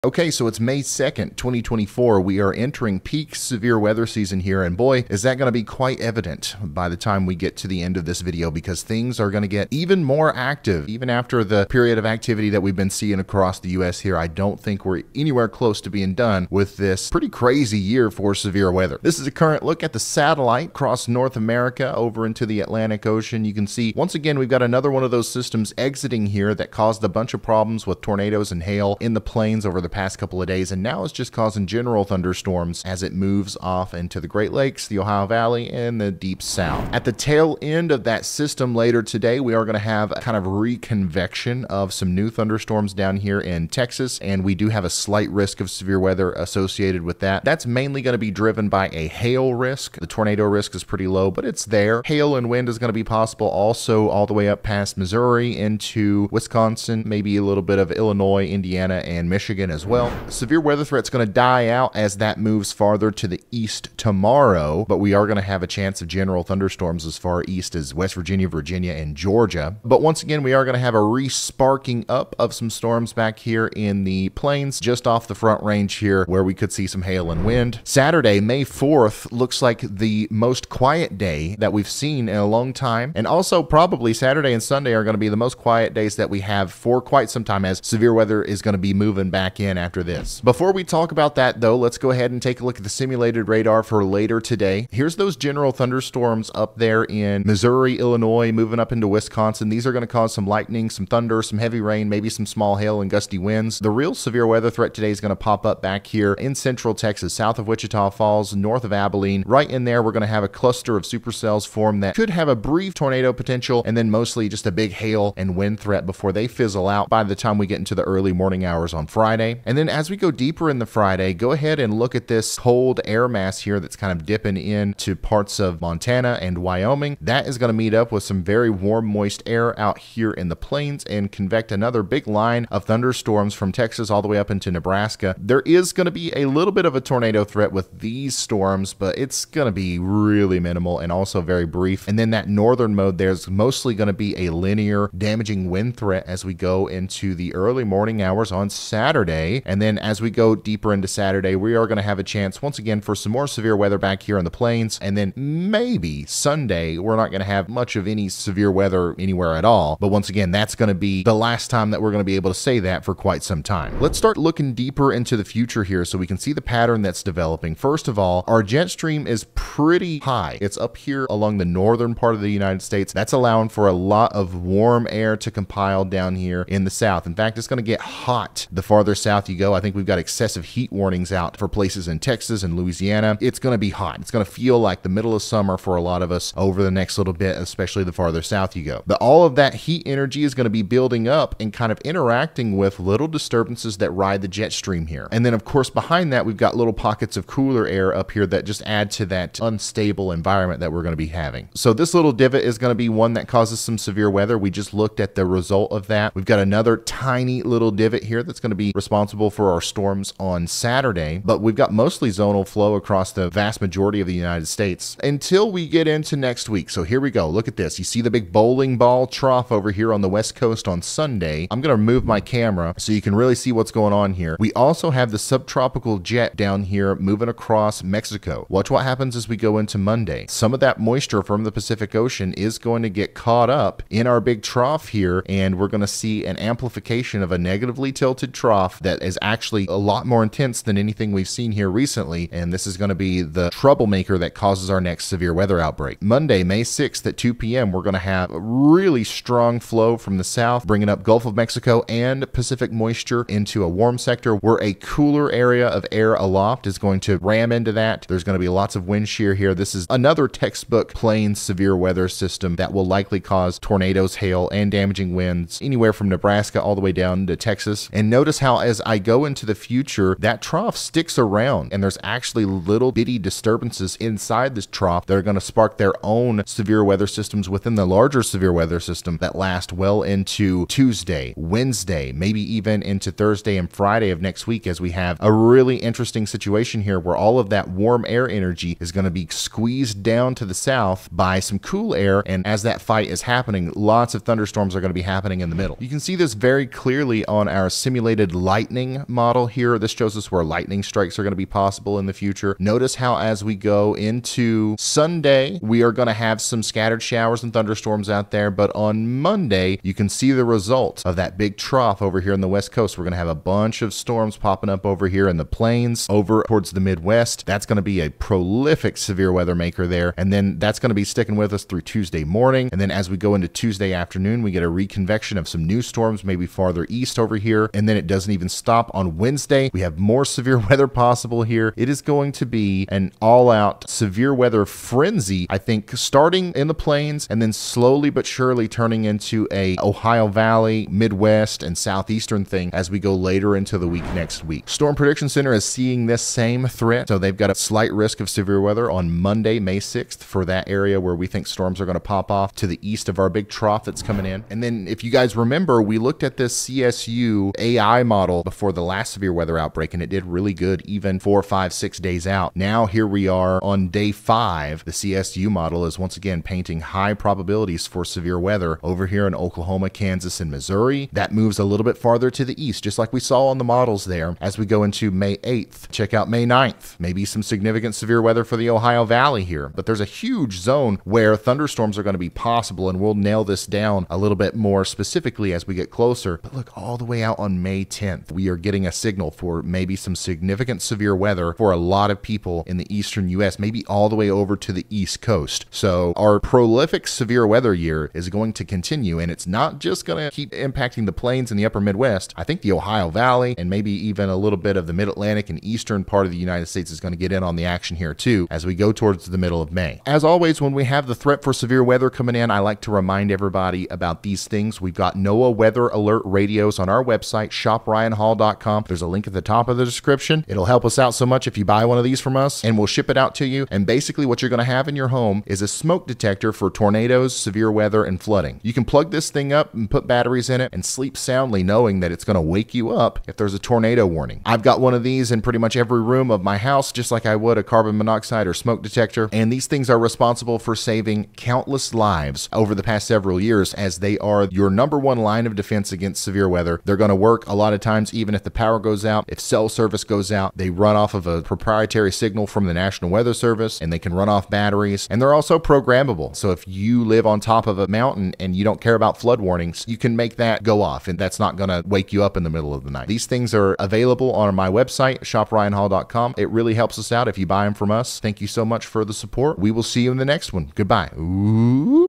Our Weather Pattern Is About To Go Nuts…
In this video we are talking about even more severe weather outbreaks in the near future…

Send us something in the mail!

Please refer to your local National Weather Service office to get the most reliable and up to date weather information as possible.